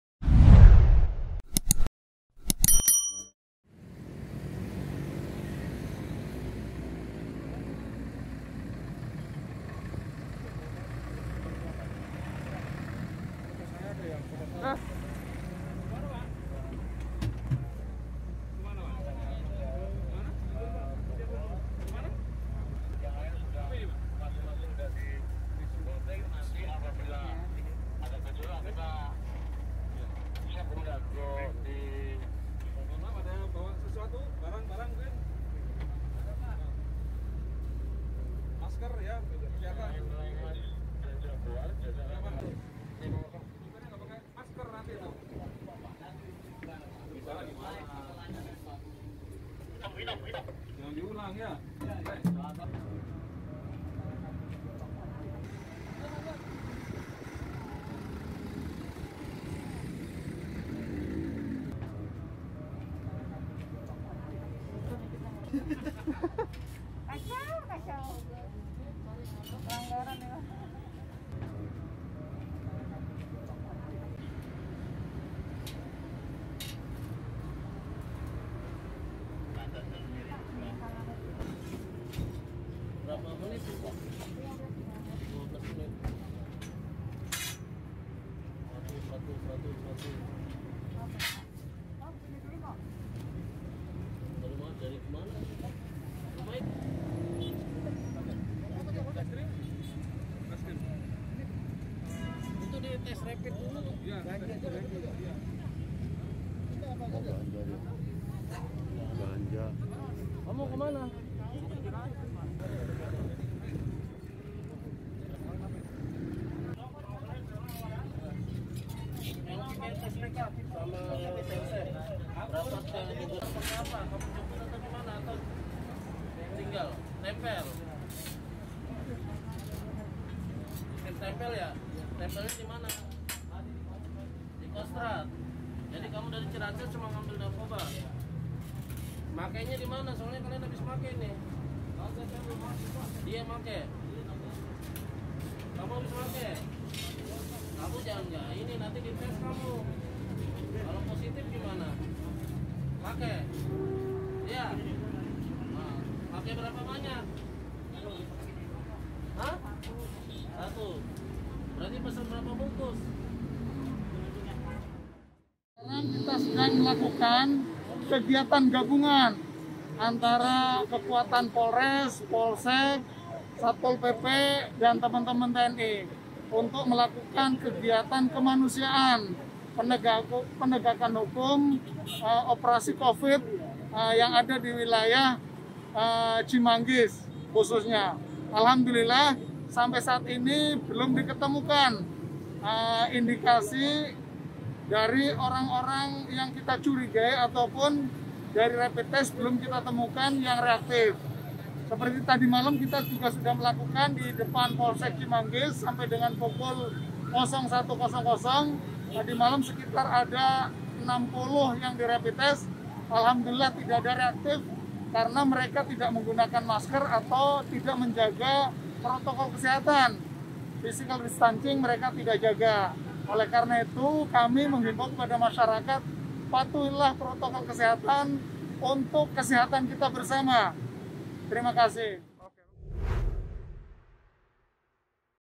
OPERASI KEMANUSIAAN DI DEPOK, POLISI AMANKAN DUA PELAJAR MEMBAWA NARKOBA
Polres Metro Depok melakukan operasi kemanusiaan di Jalan Raya Bogor (Bogor 8), Sukmajaya Kota Depok, Jumat (18/12/2020) sore. Petugas langsung melakuan rapid tes kepada para pengendara yang akan mau menuju Jakarta.

Kapolsek Cimanggis AKP Agus Khaeron mengatakan pihaknya gabungan bersama anggota Polres Metro Depok dan Sukmajaya beserta TNI melakukan penjagaan di pintu masuk Depok keluar pintu tol Cijago (Bogor 8).

"Operasi kemanusiaan kita gelar mulai usai solat Jumat. Bus angkutan umum dan kendaraan pribadi mau ke Jakarta kita periksa,"ujarnya kepada poskota di lokasi.